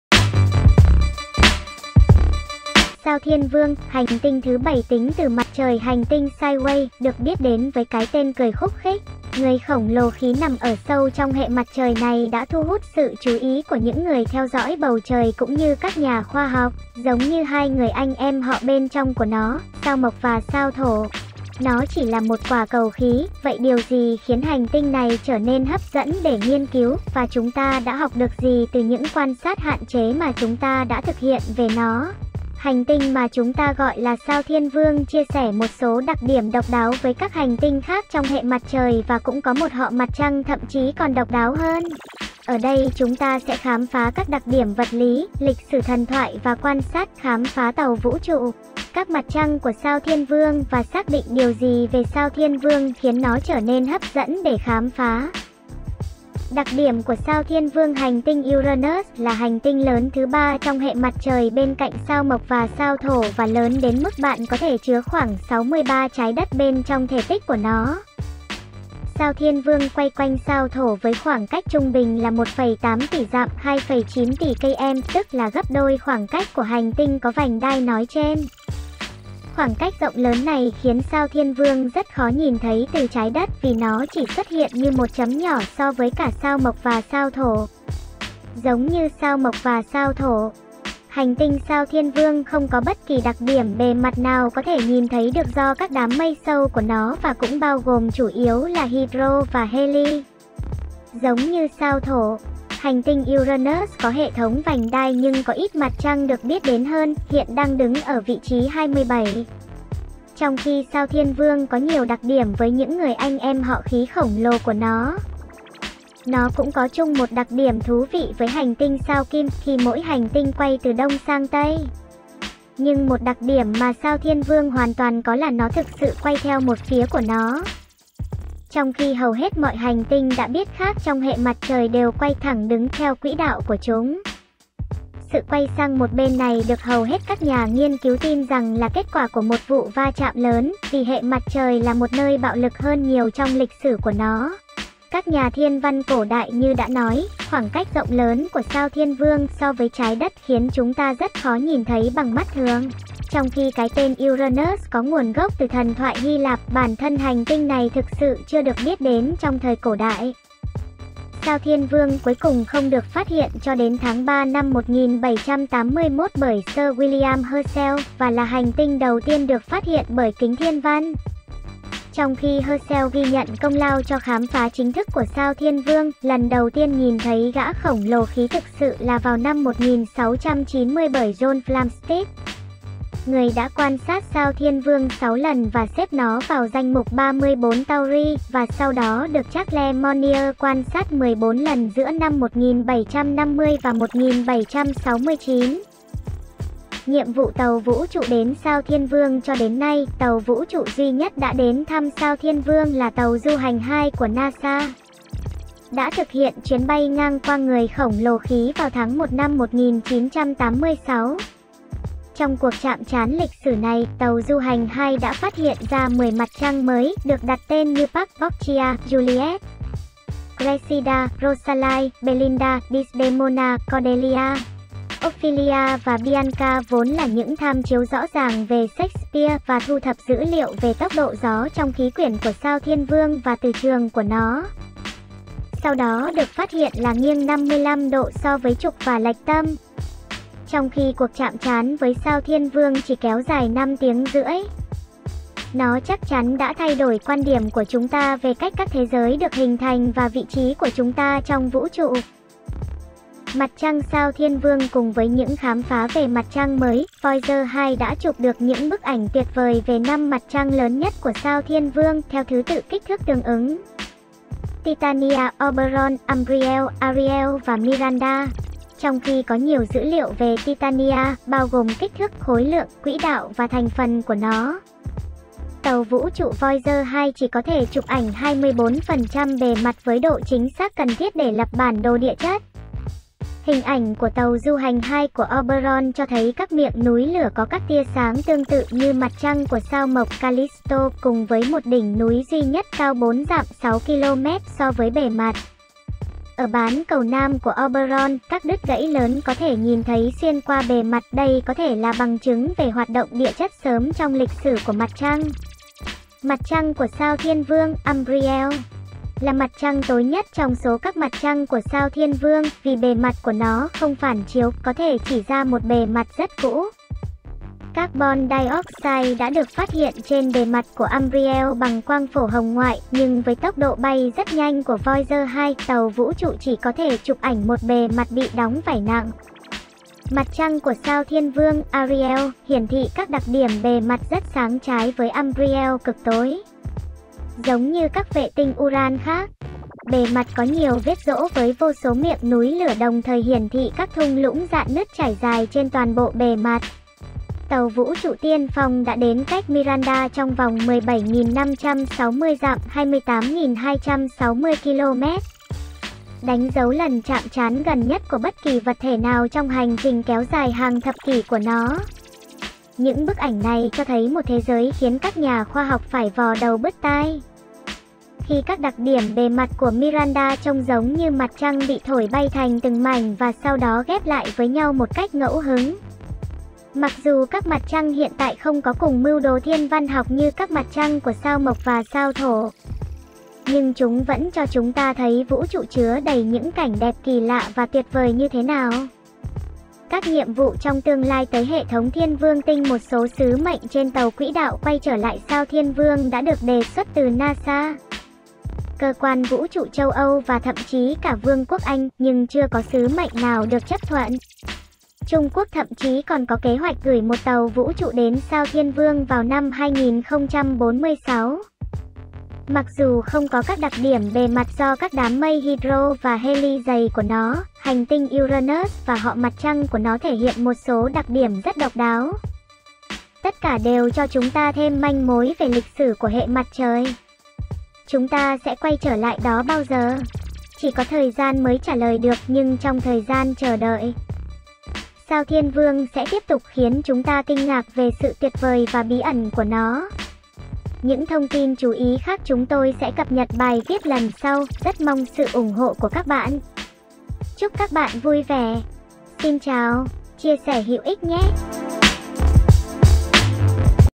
Sao Thiên Vương Hành tinh Hai bên
Mặt trăng sao Thiên vương

Cùng với những khám phá về mặt trăng mới, Voyager 2 đã chụp được những bức ảnh tuyệt vời về năm mặt trăng lớn nhất của Sao Thiên Vương (theo thứ tự kích thước tương ứng): Titania, Oberon, Umbriel, Ariel và Miranda.

Trong khi có nhiều dữ liệu về Titania , bao gồm kích thước, khối lượng, quỹ đạo và thành phần của nó, tàu vũ trụ Voyager 2 chỉ có thể chụp ảnh 24% bề mặt với độ chính xác cần thiết để lập bản đồ địa chất.

Hình ảnh của tàu du hành 2 của Oberon cho thấy các miệng núi lửa có các tia sáng tương tự như mặt trăng của Sao Mộc, Callisto, cùng với một đỉnh núi duy nhất cao 4 dặm (6 km) so với bề mặt. Ở bán cầu nam của Oberon, các đứt gãy lớn có thể nhìn thấy xuyên qua bề mặt, đây có thể là bằng chứng về hoạt động địa chất sớm trong lịch sử của mặt trăng.

Mặt trăng của Sao Thiên Vương, Umbriel , là mặt trăng tối nhất trong số các mặt trăng của Sao Thiên Vương, vì bề mặt của nó không phản chiếu, có thể chỉ ra một bề mặt rất cũ. Carbon dioxide đã được phát hiện trên bề mặt của Umbriel bằng quang phổ hồng ngoại, nhưng với tốc độ bay rất nhanh của Voyager 2, tàu vũ trụ chỉ có thể chụp ảnh một bề mặt bị đóng vảy nặng.

Mặt trăng của Sao Thiên Vương, Ariel , hiển thị các đặc điểm bề mặt rất sáng trái với Umbriel cực tối. Giống như các vệ tinh Uran khác, bề mặt có nhiều vết rỗ với vô số miệng núi lửa đồng thời hiển thị các thung lũng rạn nứt trải dài trên toàn bộ bề mặt.

Tàu vũ trụ tiên phong đã đến cách Miranda trong vòng 17.560 dặm (28.260 km) , đánh dấu lần chạm trán gần nhất của bất kỳ vật thể nào trong hành trình kéo dài hàng thập kỷ của nó. Những bức ảnh này cho thấy một thế giới khiến các nhà khoa học phải vò đầu bứt tai, khi các đặc điểm bề mặt của Miranda trông giống như mặt trăng bị thổi bay thành từng mảnh và sau đó ghép lại với nhau một cách ngẫu hứng.

Mặc dù các mặt trăng hiện tại không có cùng mưu đồ thiên văn học như các mặt trăng của Sao Mộc và Sao Thổ, nhưng chúng vẫn cho chúng ta thấy vũ trụ chứa đầy những cảnh đẹp kỳ lạ và tuyệt vời như thế nào.

Các nhiệm vụ trong tương lai tới Hệ thống Thiên Vương tinh

Một số sứ mệnh trên tàu quỹ đạo quay trở lại Sao Thiên Vương đã được đề xuất từ ​​NASA , Cơ quan Vũ trụ Châu Âu và thậm chí cả Vương quốc Anh , nhưng chưa có sứ mệnh nào được chấp thuận. Trung Quốc thậm chí còn có kế hoạch gửi một tàu vũ trụ đến Sao Thiên Vương vào năm 2046.

Mặc dù không có các đặc điểm bề mặt do các đám mây hydro và heli dày của nó, hành tinh Uranus và họ mặt trăng của nó thể hiện một số đặc điểm rất độc đáo, tất cả đều cho chúng ta thêm manh mối về lịch sử của hệ mặt trời. chúng ta sẽ quay trở lại đó bao giờ? Chỉ có thời gian mới trả lời được, nhưng trong thời gian chờ đợi, sao Thiên Vương sẽ tiếp tục khiến chúng ta kinh ngạc về sự tuyệt vời và bí ẩn của nó.